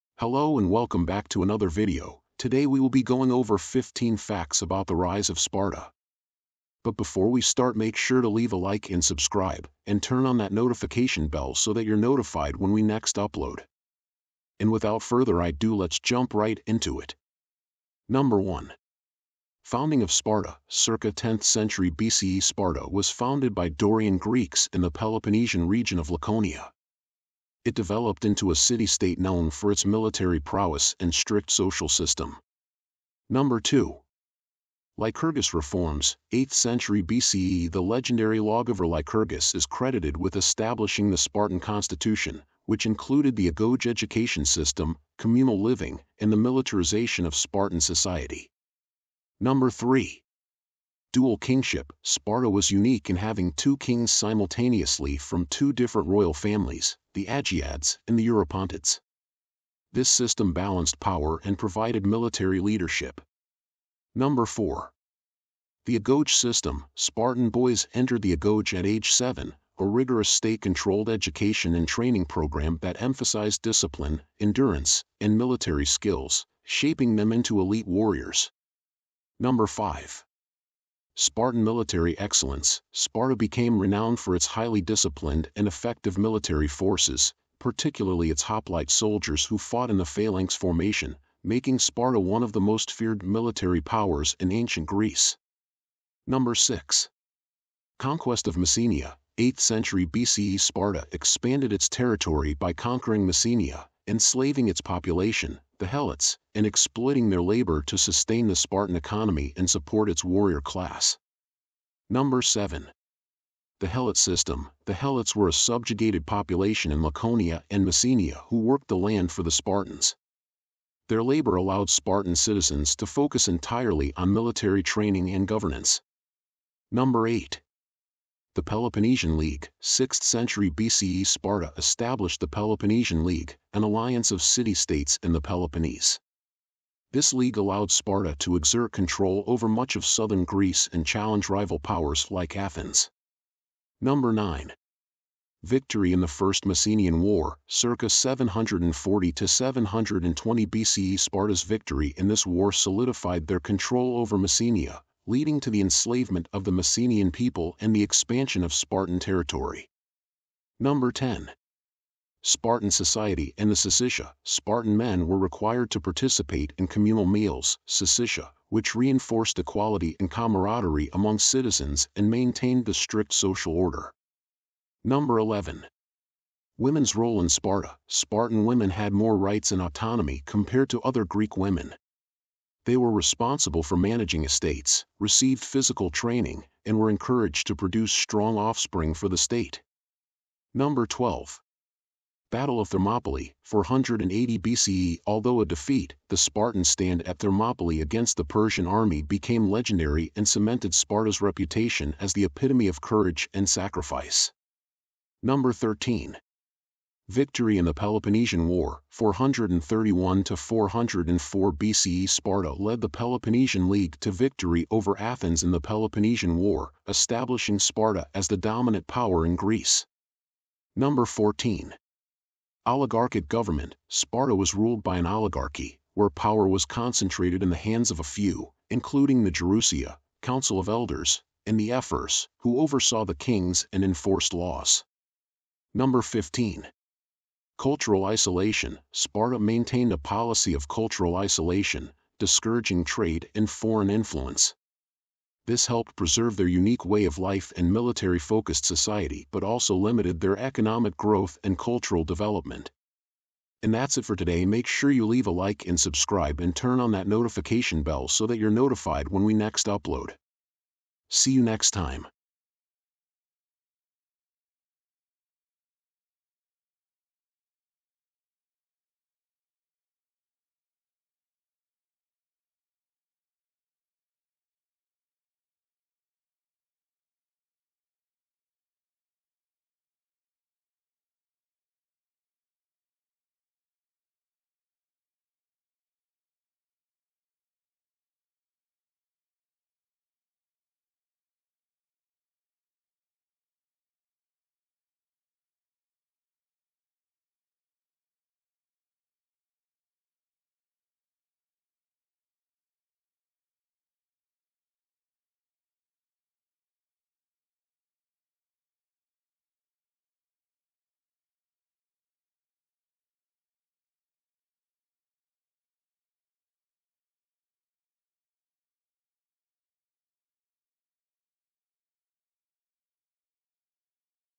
15 Facts About The Rise Of Sparta
In this video we will be going over 15 Facts About The Rise Of Sparta.